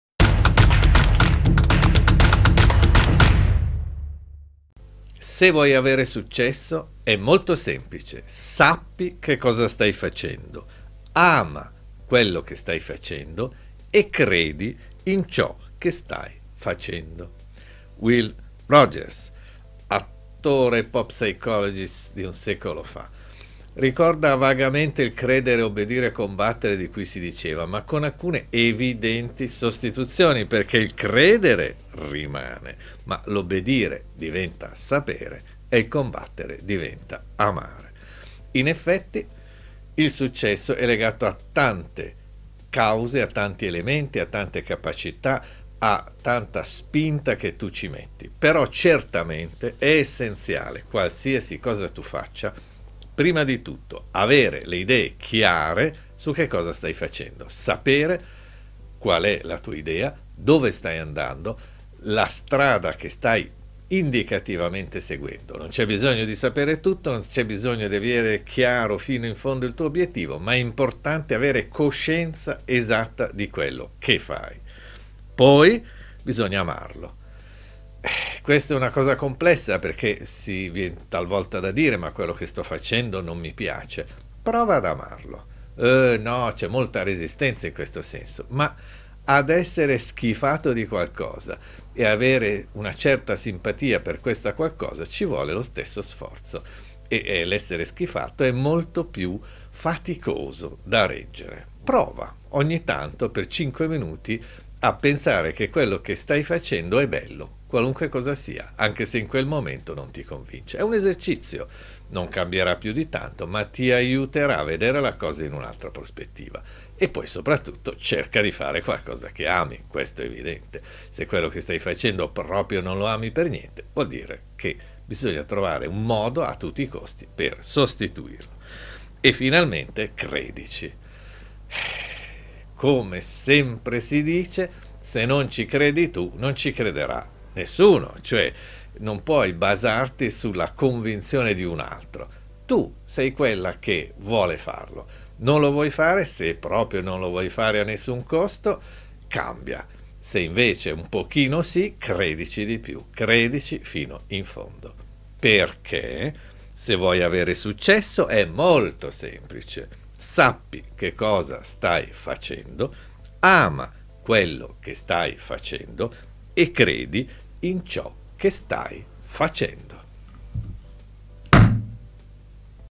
Se vuoi avere successo ...
Se vuoi avere successo, è molto semplice. Sappi che cosa stai facendo. Ama quello che stai facendo. E credi in ciò che stai facendo
@ Will Rogers

PS: Per qualche oscura ragione che mi sfugge (temo però che sia stata una mia distrazione) la didascalia, che solitamente vaga nei video di Esserci, qui è molto imprecisa e sostanzialmente inutile.

Me ne scuso, ma non mi pare elegante sostituire il video. Si vede che era destino.

Quindi: grazie dell'attenzione. E cercherò di riscattarmi per il futuro. In alto i cuori!

 Per attingere a tutte le citazioni che ci ispirano ad ESSERCI sempre al meglio, vai subito alla playlist della Serie: